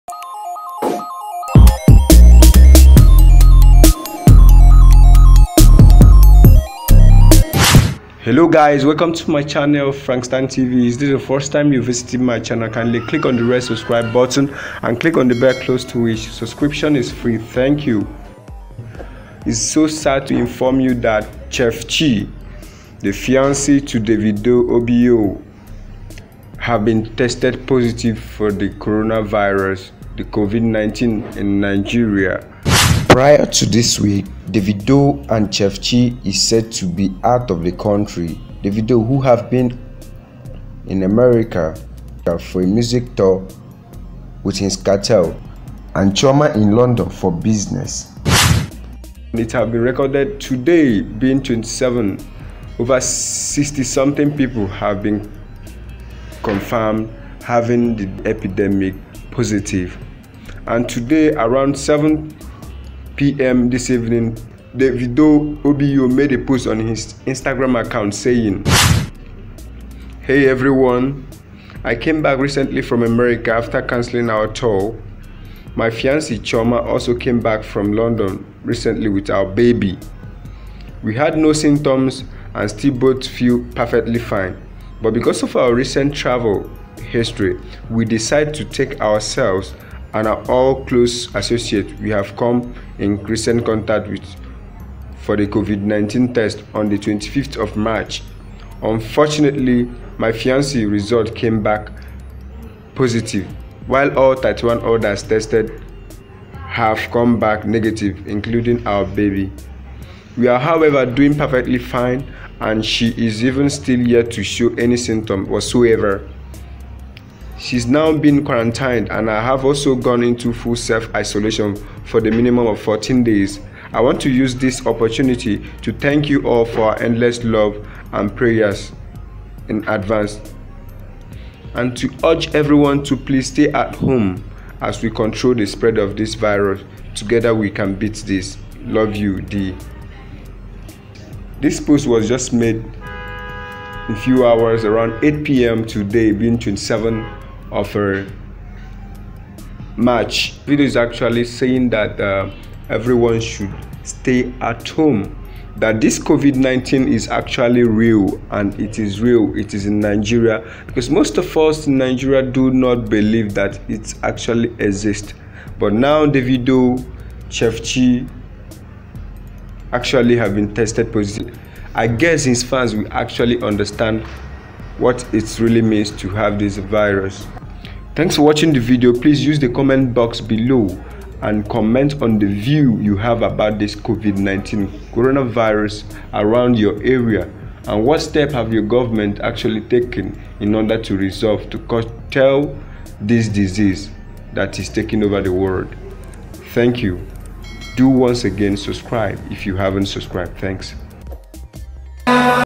Davido Fiance Choma To Be Tested
Davido just confirmed chef chioma,  His Fiance has
HE ADVISE EVERY ONE TO STAY SAFE THAT XCORONA VIRUS IS REAL BE PRECAUSCIOUS